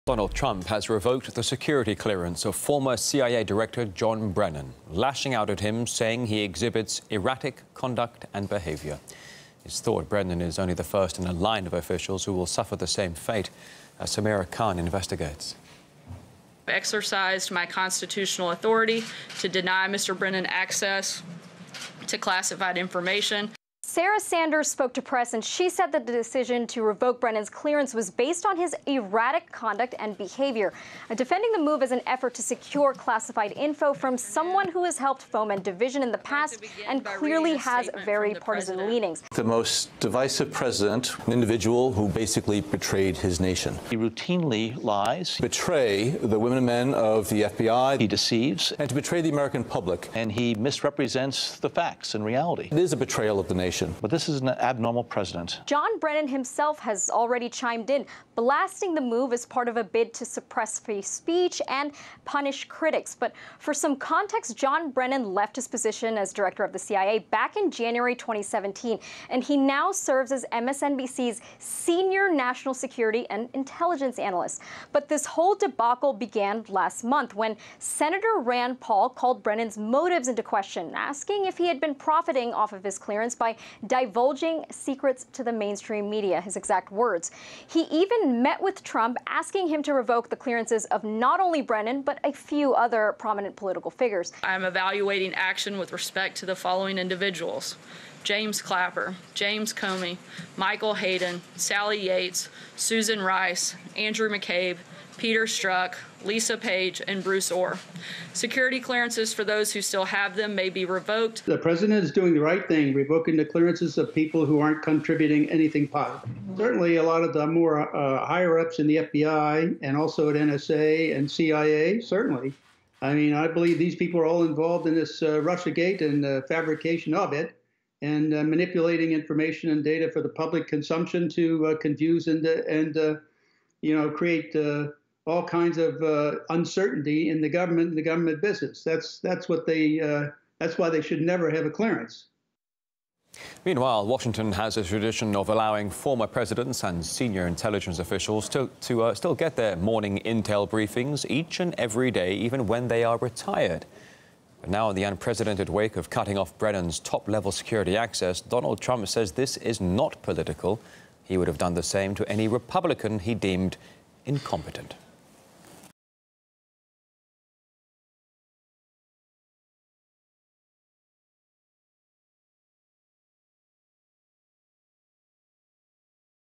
'Brennan's Erratic Conduct & Behavior' Trump Revokes Security Clearance Of Ex CIA Chief
August 16, 2018 - Donald Trump has revoked the security clearance of former CIA Director, John Brennan lashing out at him, saying he exhibits 'erratic conduct & behaviour.' And it's thought Brennan is only the first in a line of officials who will suffer the same fate.